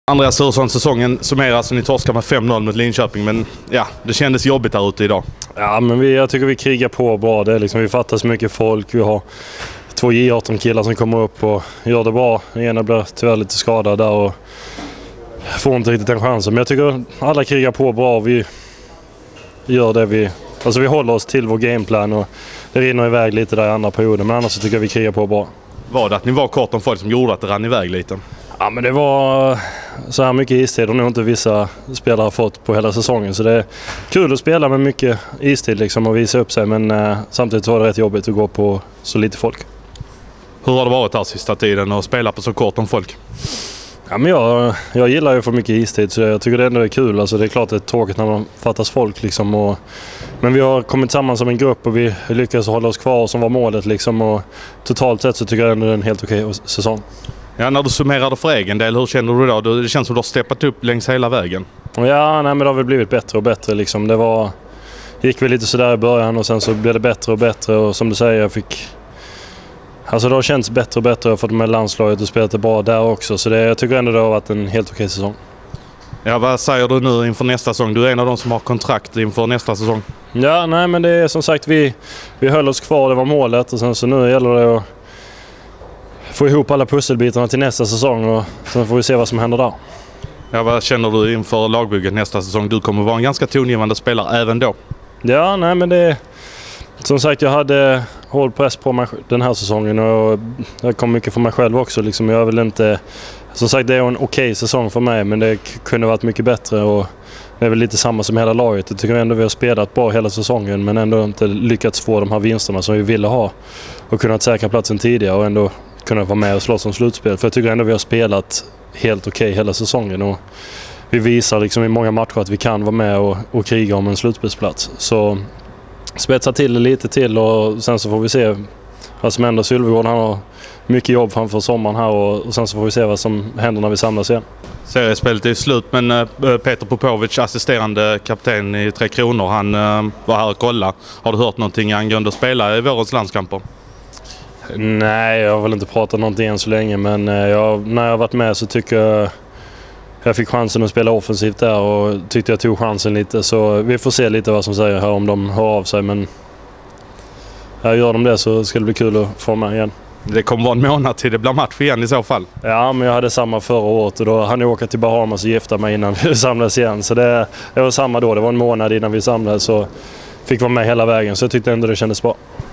MrMadhawk.se Thuresson efter LHC-Malmö 2016-03-08
Malmös Andreas Thuresson efter att man spelat klart för säsongen i Linköping.